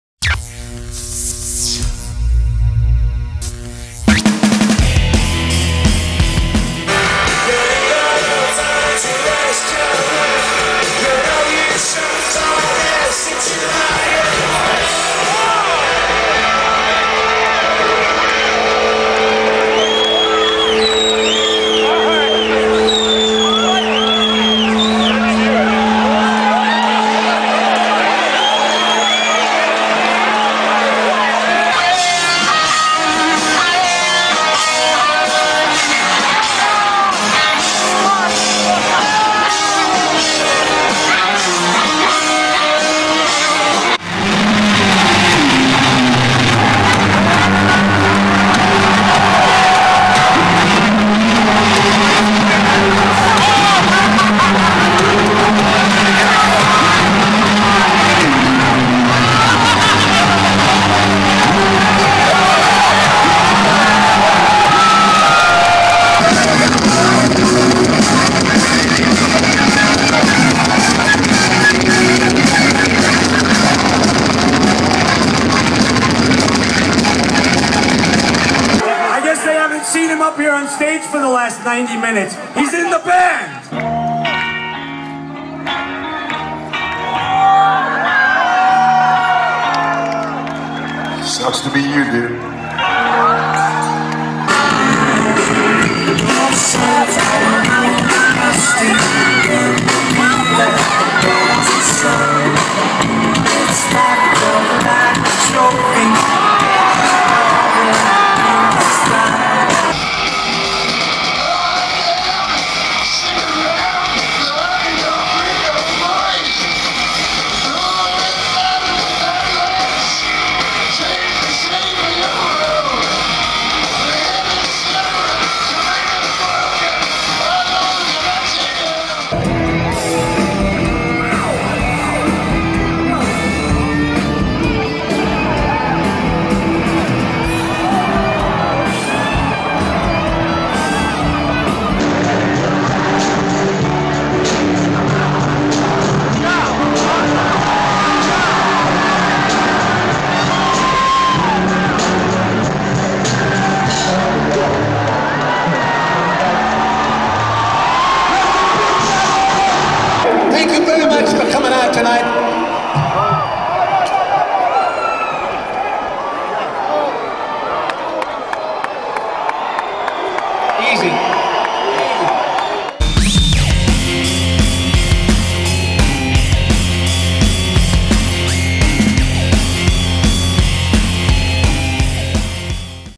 10 Stage Crashers Getting Smashed by Security
We’ve all seen moments when concert security get a little too rough with fans, but other times, security guards need to step in to protect an artist.